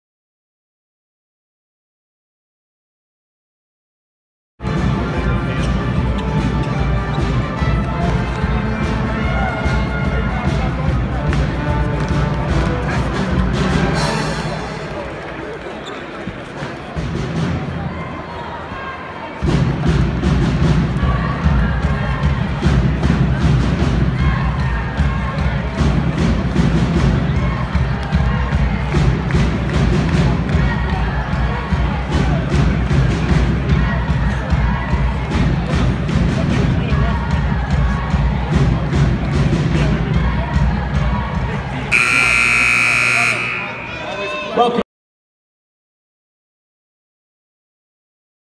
Bacari Alexander and LaVall Jordan meet at halfcourt
Detroit Mercy vs Milwaukee at Calihan Hall